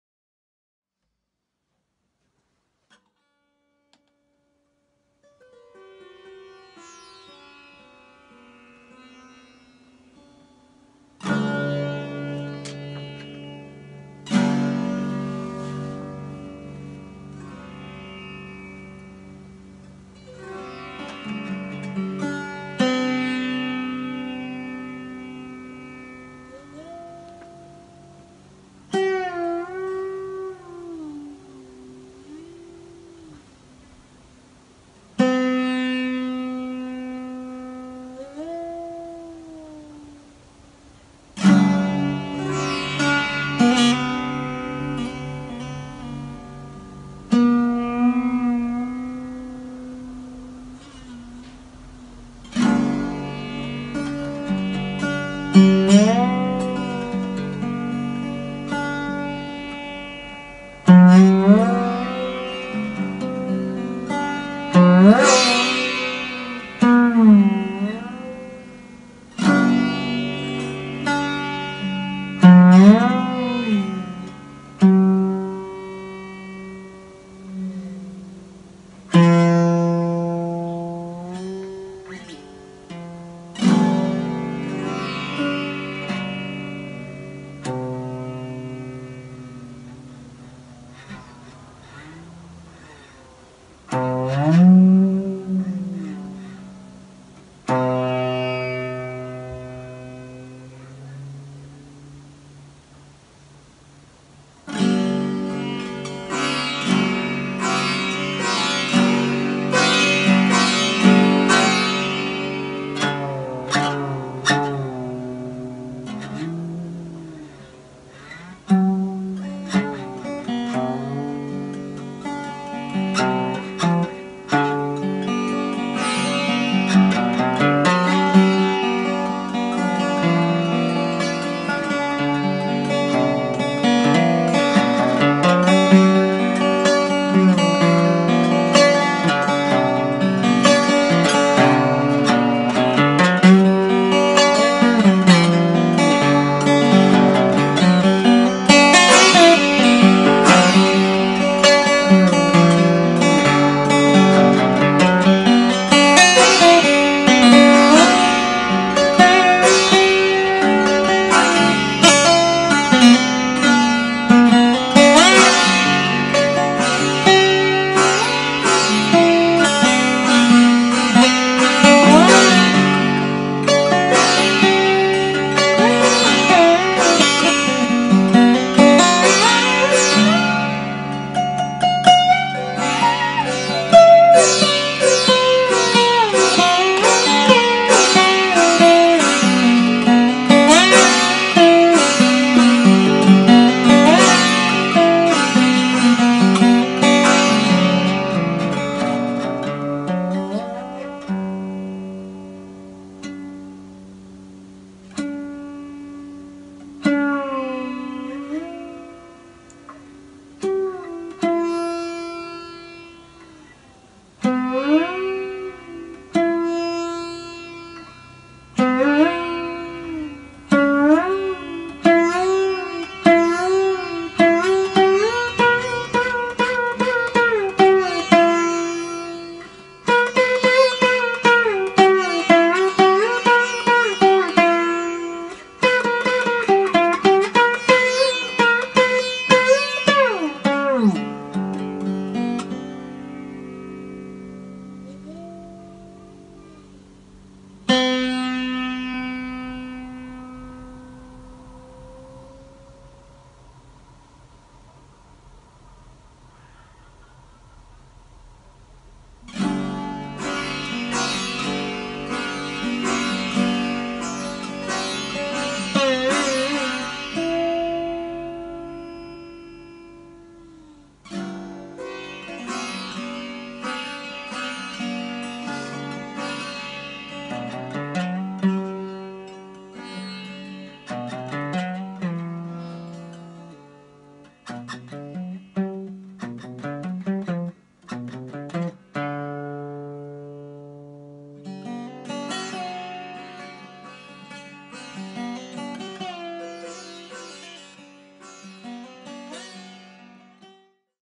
Crystal Collaboration with Shivam Rath on the Crystal Slide & Kyoko Aoyama's Crystal Art
Music based on Raga Jogeshwari with the Crystal Ball on the Indian Slide Guitar by Shivam Rath from Australia. Artwork from the ancient Crystal Painting technique with goddess art by Kyoko Aoyama from Japan.